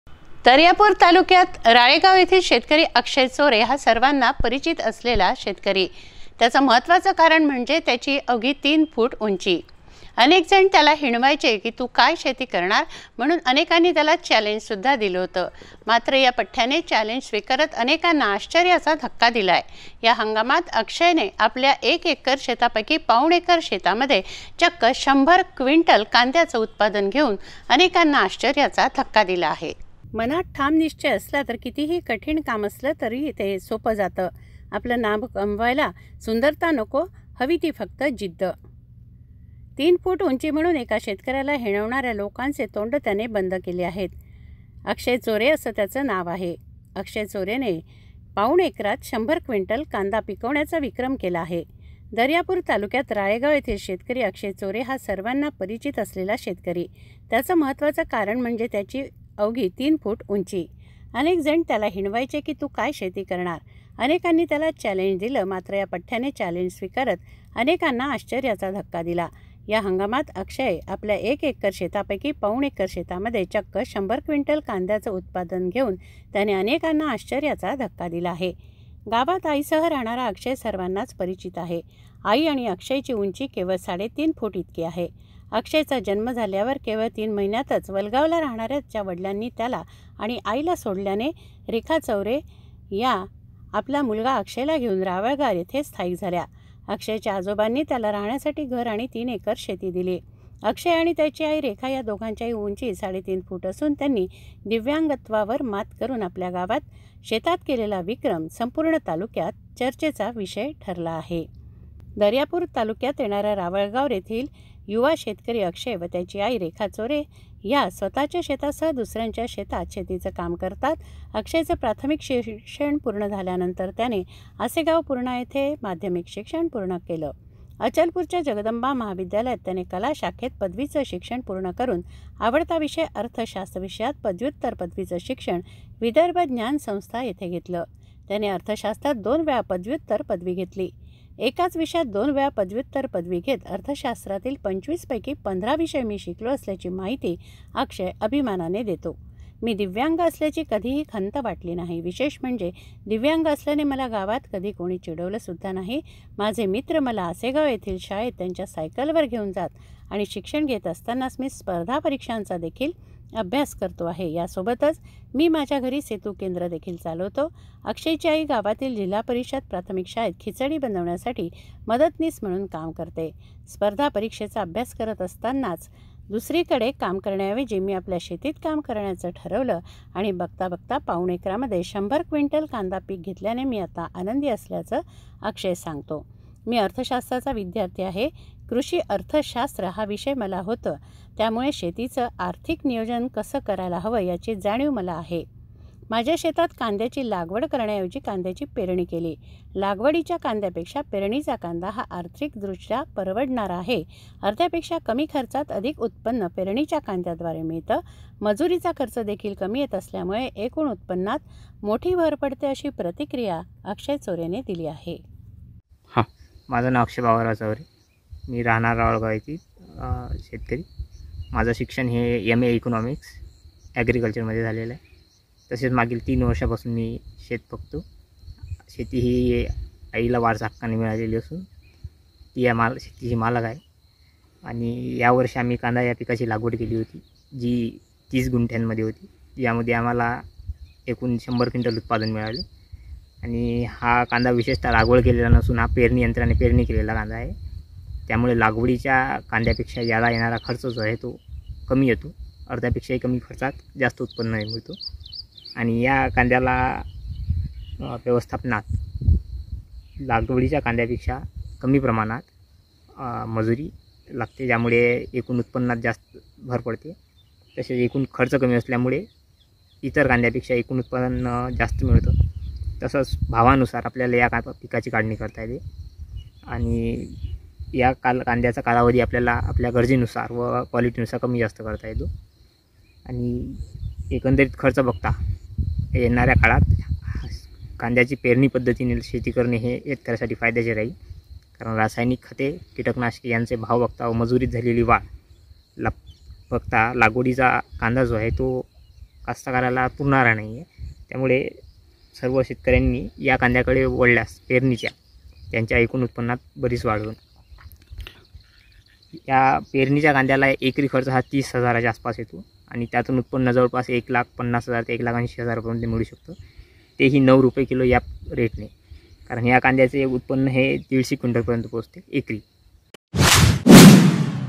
#'मूर्तीलहानकीर्ती_महान' चा परिचय दिला शेतकऱ्याने
पाऊण एकरात पिकवला
तीन फूट उंची असल्याने कमी लेखत होते सगळे